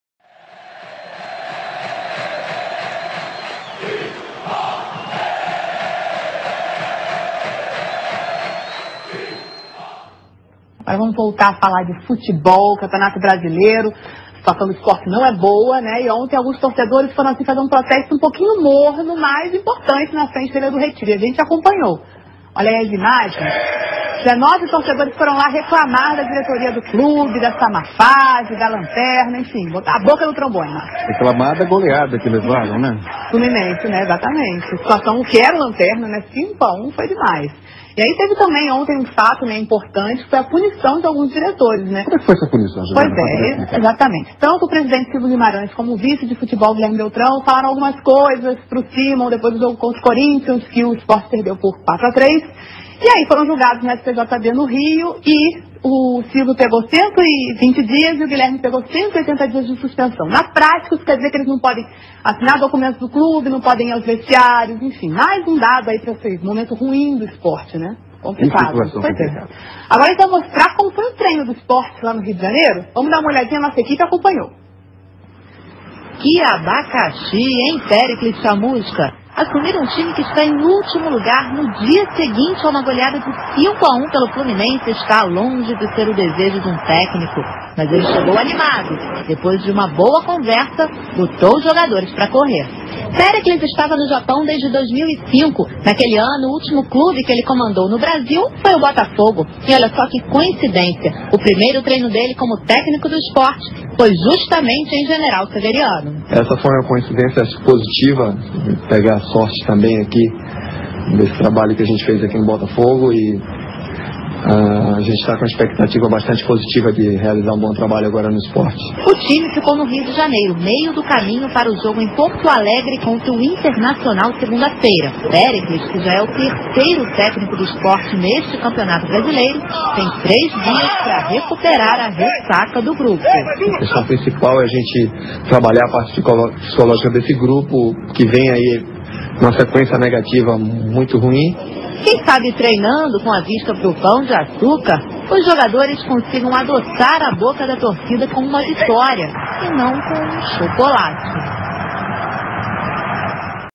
Multimídia SCR#: Chamusca promete trabalhar lado psicológico dos jogadores
Para Péricles Chamusca, que foi contratado para comandar o Sport, é hora de tocar a bola pra frente.

Membros da diretoria são suspensos, e torcida protesta na sede